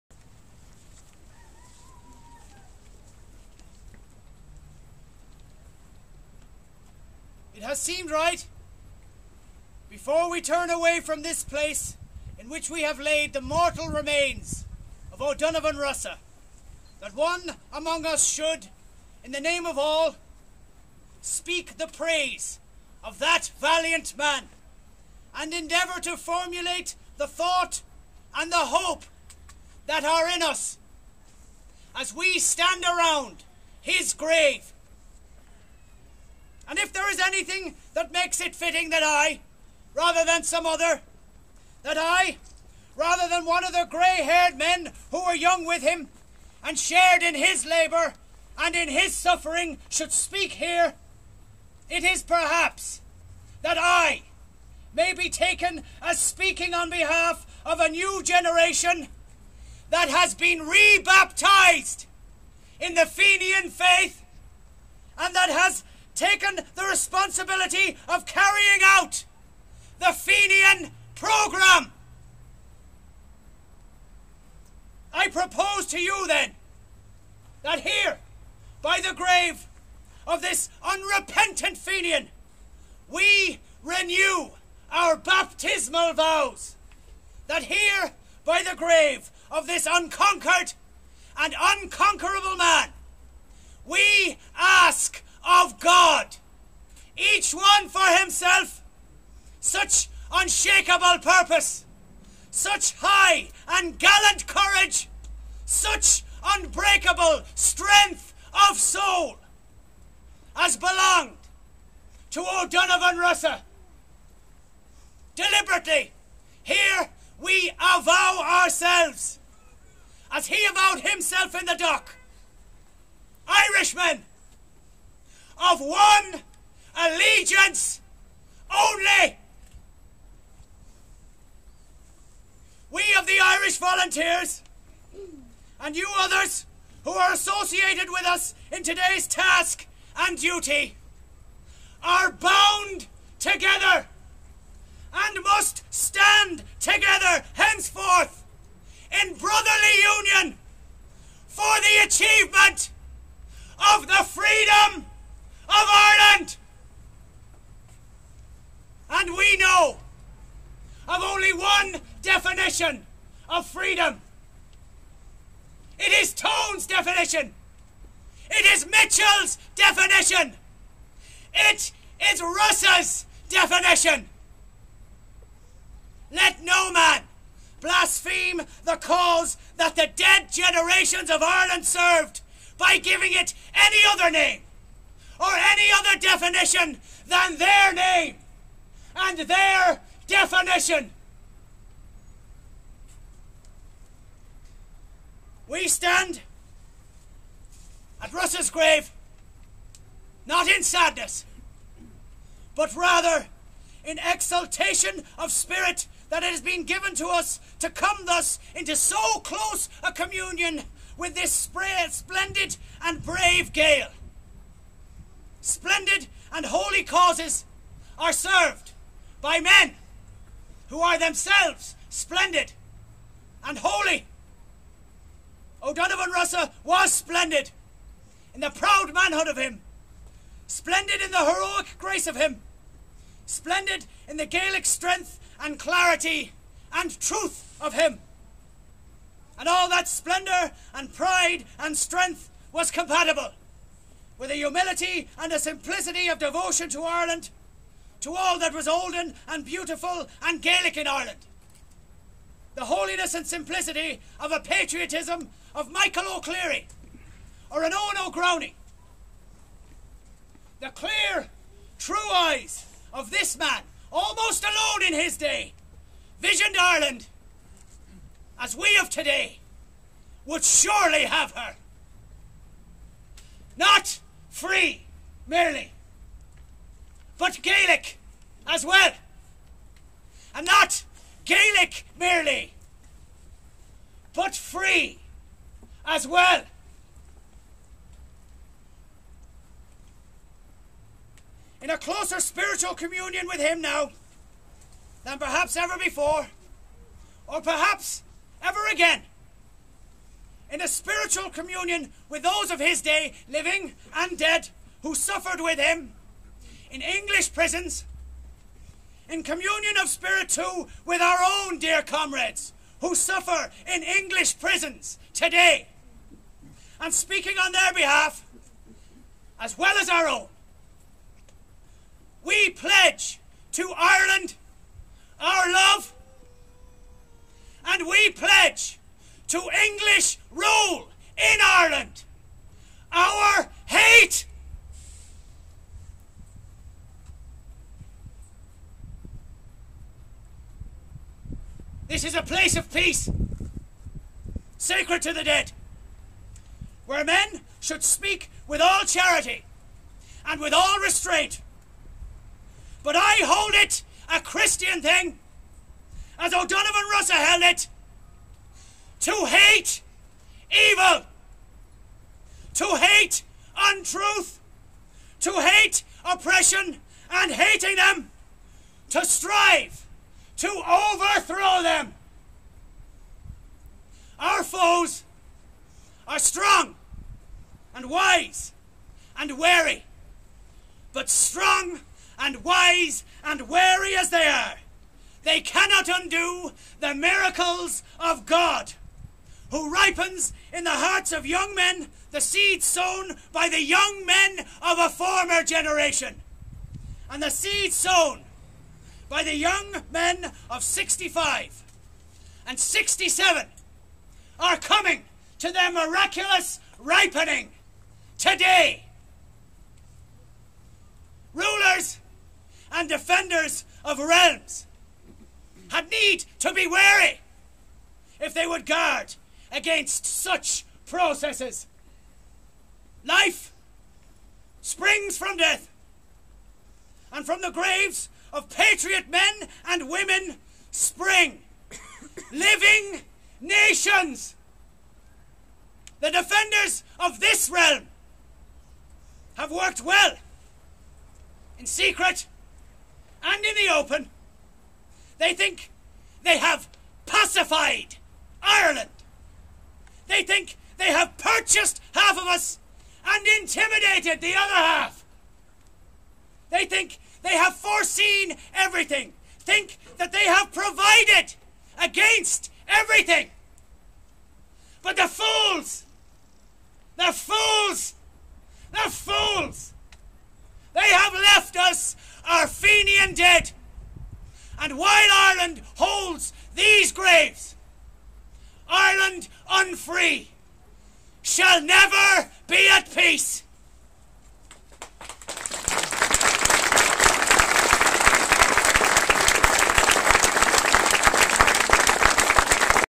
Re-enacment of Padraig Pearse's speech at O'Donovan Rossa's funeral 1915
Re-enacment 4th Aug 2014 of Padraic Pearse's graveside oration at O'Donovan Rossa's funeral in Glasnevin Cemetery